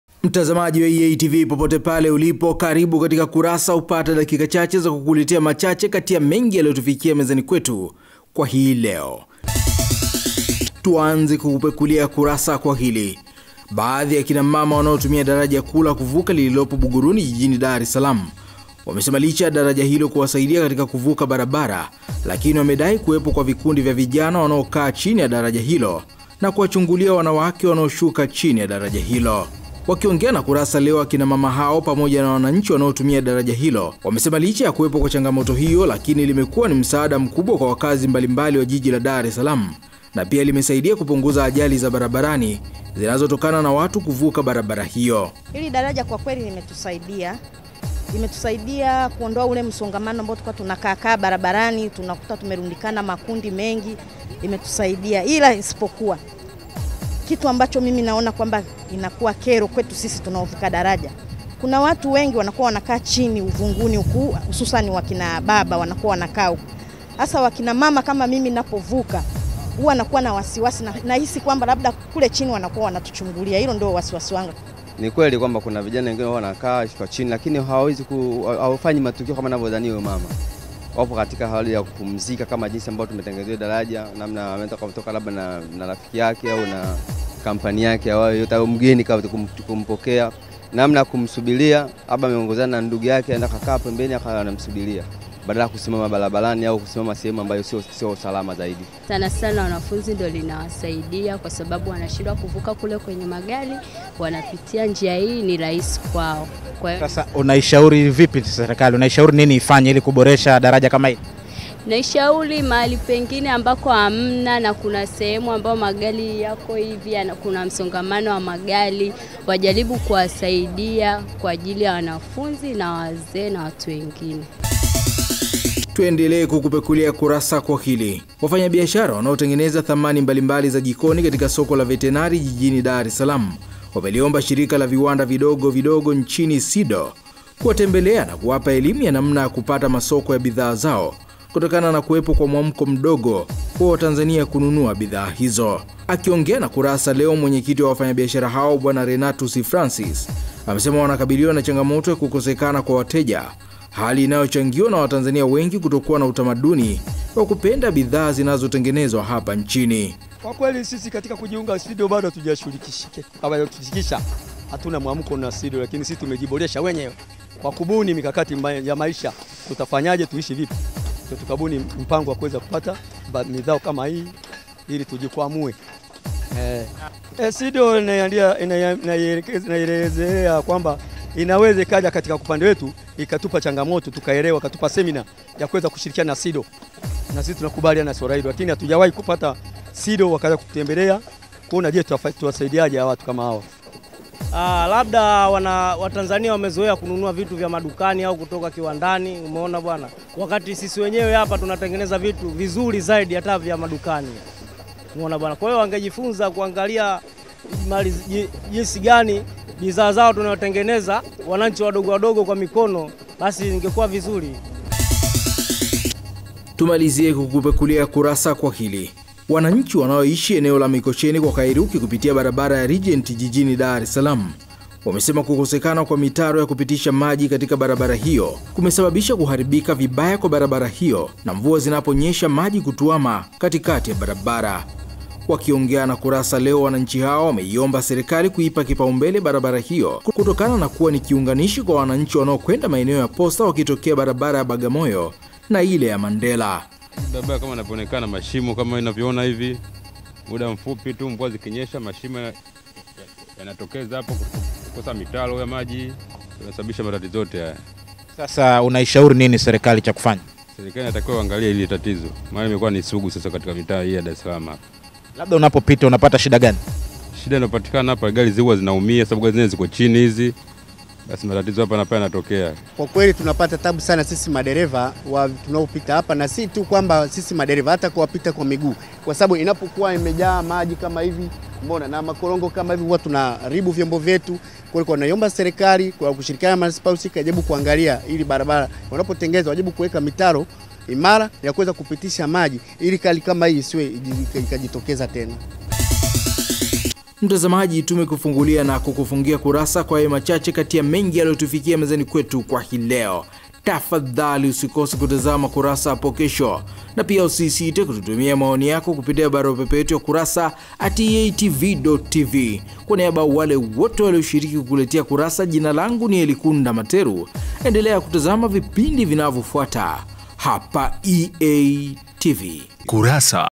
KURASA - WAKINA MAMA WANAOVUKA BARABARA DARAJA LA BUGURUNI WALALAMIKIA
Baadhi ya akina mama wanaotumia daraja la juu kuvuka lililopo Buguruni jijini Dar es salaam wamesema licha ya daraja hilo kuwasaidia katika kuvuka barabara lakini wamedai kuwepo kwa vikundi vya vijana wanaokaa chini ya daraja hilo na kuwachungulia wanawake wanaoshuka chini ya daraja hilo